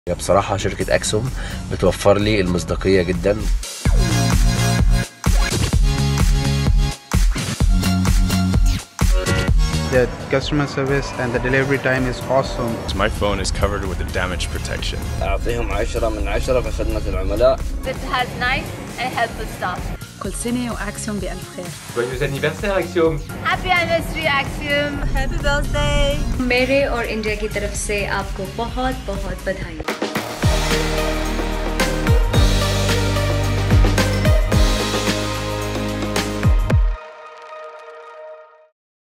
Axiom 20th Anniversary
Thank you to our loyal customers for making our journey possible! Here’s what some of you had to say in celebration of our 20th anniversary!